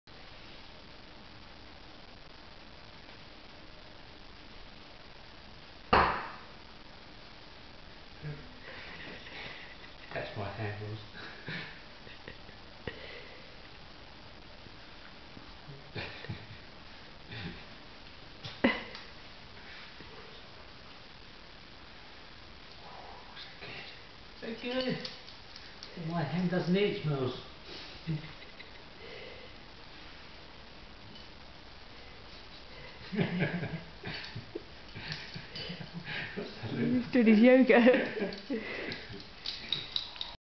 "Oh yeah.. that's the spot" says Merlin the Brittany
Merlin enjoys a good scratch so much that the old back leg get involved.